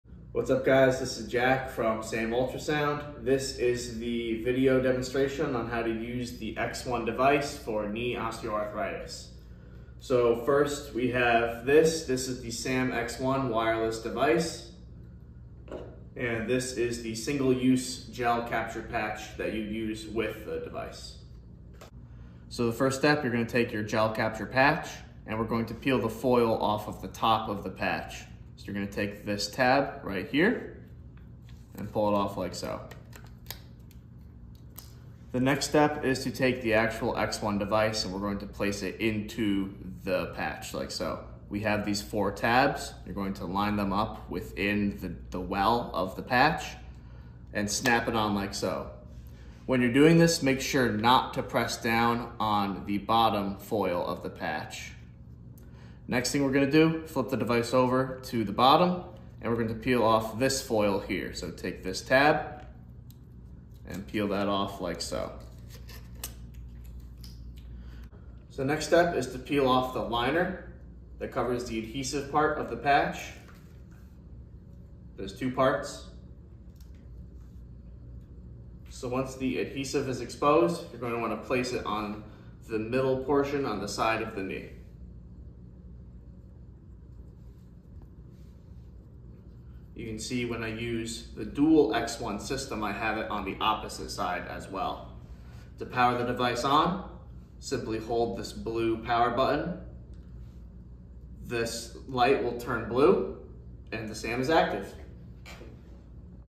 sam®X1 Demonstration for Knee Osteoarthritis Pain
Sustained Acoustic Medicine (sam®) is a low intensity, long duration, daily ultrasound treatment that can reduce the need for pain medication and joint surgery.

Using mechanobiological technology, sam® increases blood vessel diameters to improve blood flow to the knee joint. This increases oxygenated hemoglobin at the site and removes cytokine enzymes and cellular waste. The result is more rapid healing and reduced pain.